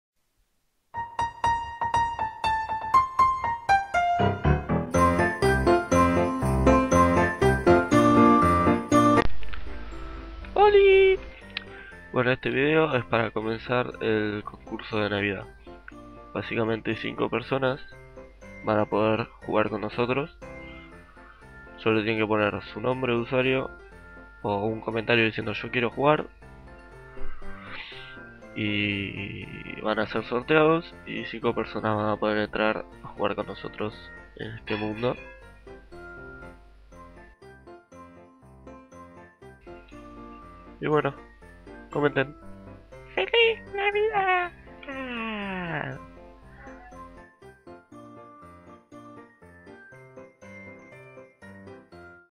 TurbiCraft - FELIZ NAVIDAD + Queres jugar con nosotros? =)
LEER ACA LEER ACA LEER ACA LEER ACA

Como dice en el video: 5 personas tendran la oportunidad de jugar con nosotros (Pr3dator Apocallypto y Yo) y algun otro amigo nuestro invitado tambien.
Tienen que dejar en los comentarios su nombre de usuario mas la frase Quiero Jugar por ejemplo: headhuntercodmw2 Quiero Jugar.

IMPORTANTE!
SOLO GENTE DE AMERICA PODRA CONECTARSE. PIDO MIL DISCULPAS A MIS SUBSCRIPTORES ESPAÑOLES PERO AL SER MUY LEJOS DE EL SERVER (ARGENTINA) LA CONEXION SERIA MUY MALA PARA USTEDES CAUSANDO QUE: O NO PUEDAN CONECTARSE O QUE EL LAG SEA INSOPORTABLE PARA USTEDES Y NO PUEDAN HACER NINGUNA DE LAS ACTIVIDADES PLANEADAS.

YA HABRA UN REGALO PARA USTEDES TAMBIEN YA QUE TENGO OTRA SORPRESA TRABAJANDO =).

Feliz navidad y EMPIECEN A COMENTAR! El sorteo lo voy a hacer yo grabandome para que vean que es aleatorio =)

(EL QUE INTENTE PERSUADIRME O ALGO POR PM O ALGO ASI DIRECTAMENTE PIERDE LA OPORTUNIDAD DE PARTICIPAR)

Una ves que aparezca el video del sorteo dare la informacion a todos los que ganaron sobre como y cuando entrar al server =)